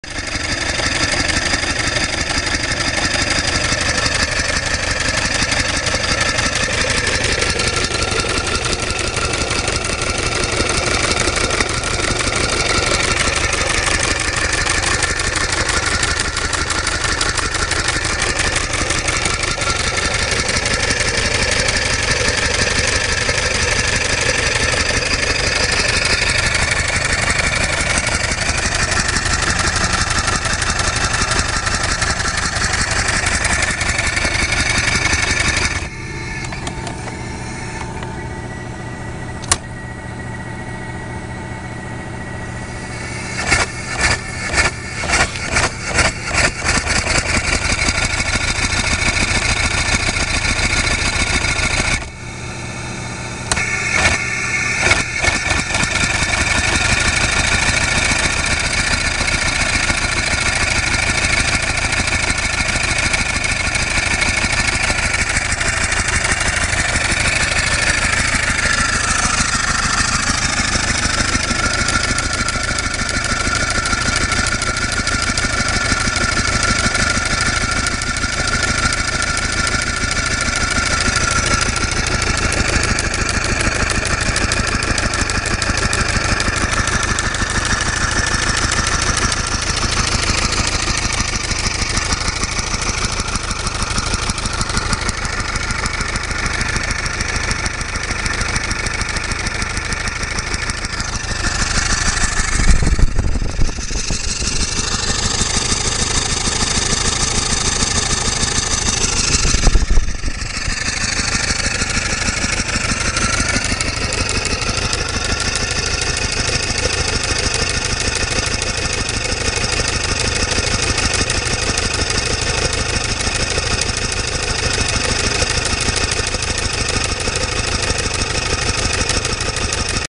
Original Stanley steamer car engine
Very nice, all original Stanley car engine running on compressed air.